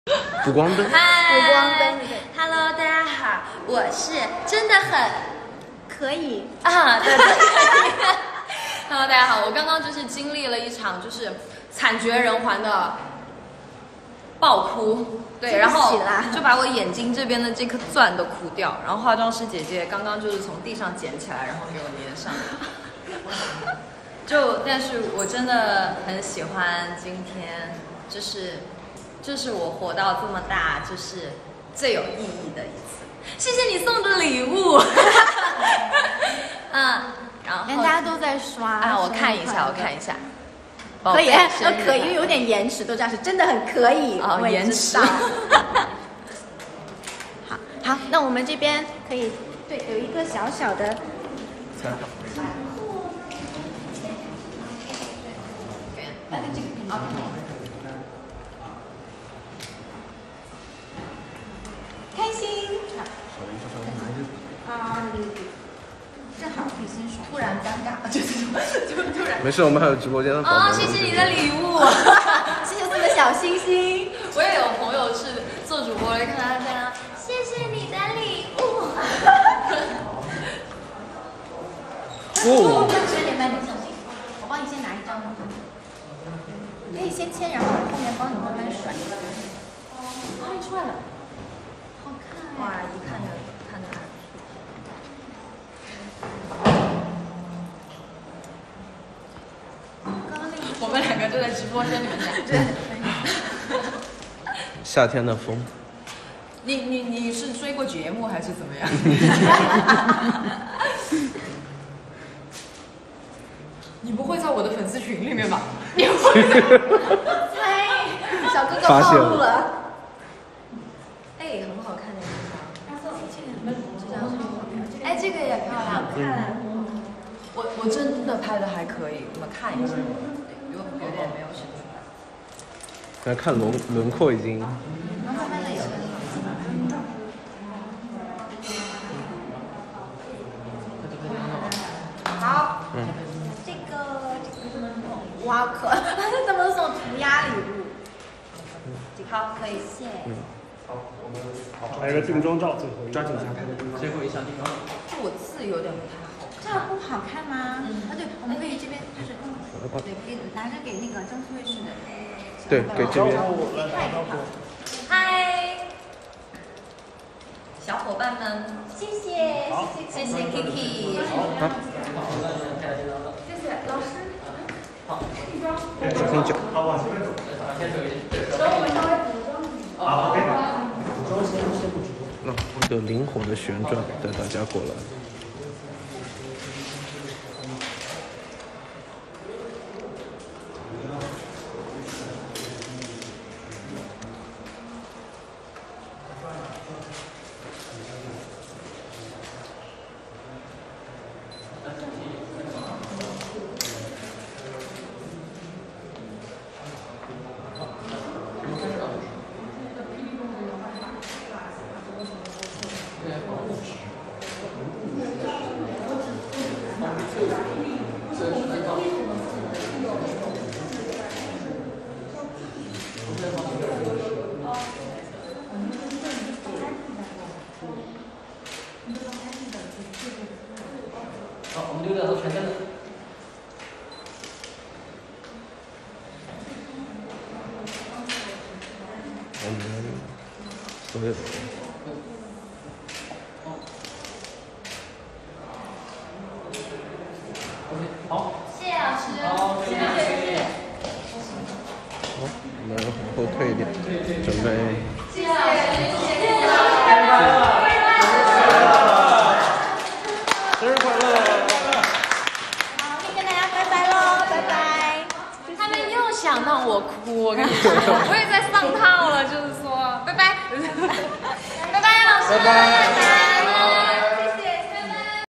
[ENG SUB] Keni TMall 618 Prerecording Livestream
Keni cut from the TMall prerecording event.

Original video from weibo user 安仔啾啾糖.